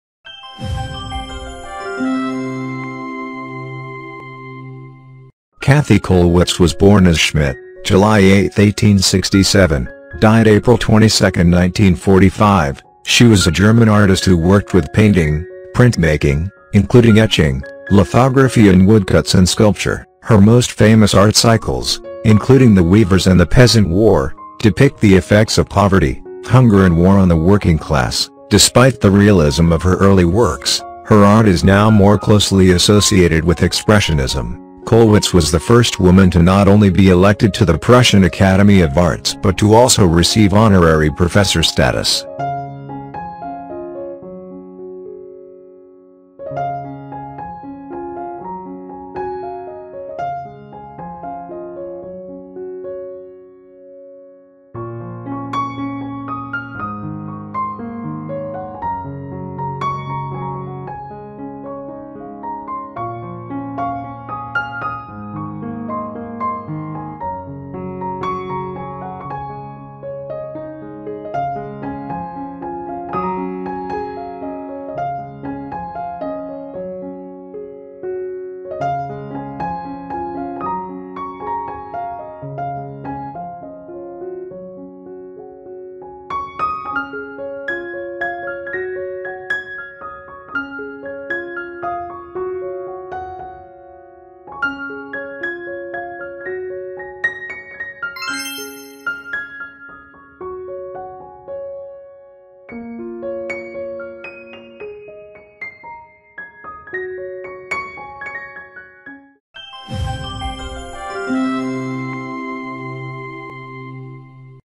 Artist Kathe Kollwitz (1867 - 1945) | German Painter | WAA
Kathe Kollwitz was born as Schmidt, 8 July 1867, died 22 April 1945, she was a German artist who worked with painting, printmaking, including etching, lithography and woodcuts and sculpture. Her most famous art cycles, including The Weavers and The Peasant War, depict the effects of poverty, hunger and war on the working class. Despite the realism of her early works, her art is now more closely associated with Expressionism. Kollwitz was the first woman to not only be elected to the Prussian Academy of Arts but to also receive honorary professor status.